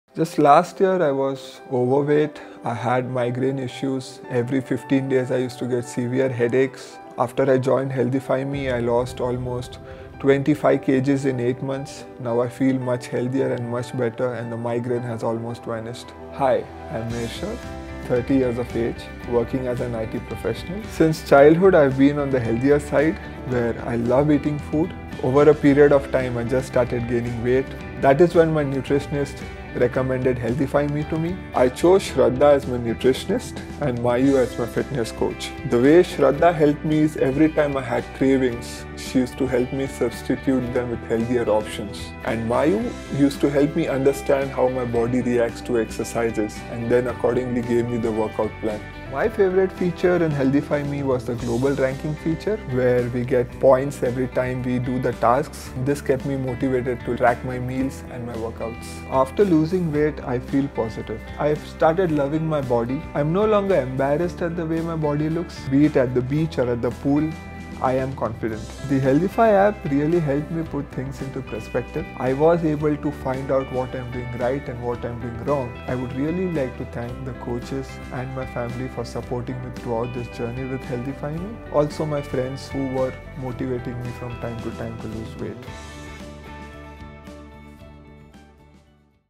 WEIGHT LOSS SUCCESS STORY - How Mershad Lost 25 Kgs In 8 Months Using HealthifyMe App
Do you want to lose weight and get into shape like Mershad? Download the HealthifyMe app today and get started on your healthy journey today
Available on Android and iOS.